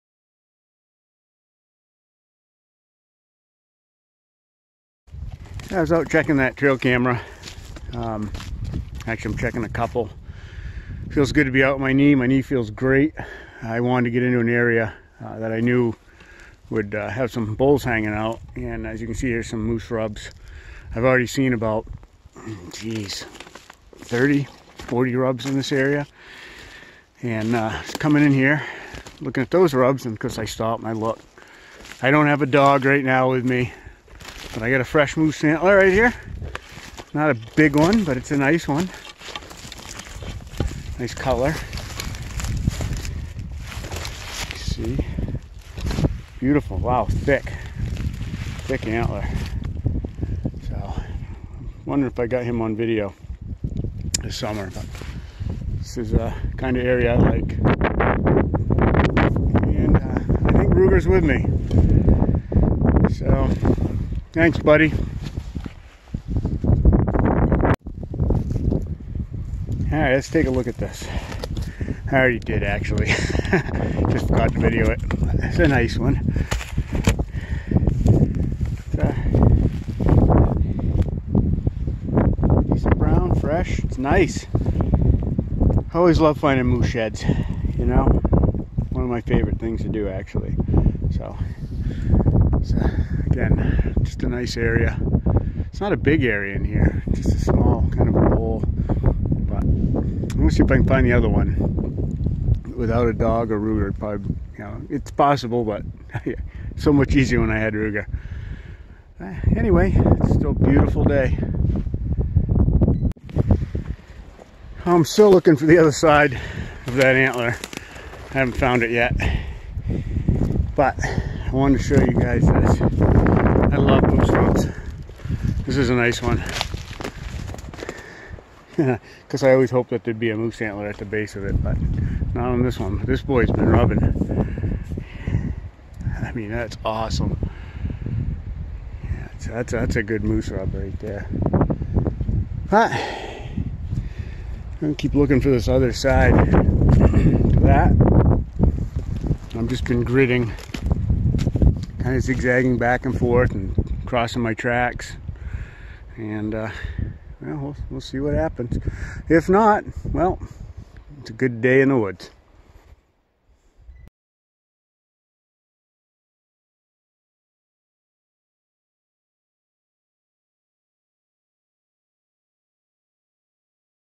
Finding a Maine Moose Shed ~ December 27, 2020
After checking a couple of my trail cameras I hiked into an area I thought Moose might be wintering. I got lucky and found a nice moose shed, my first since my shed hunting dog, Ruger passed away. This was a special one for me.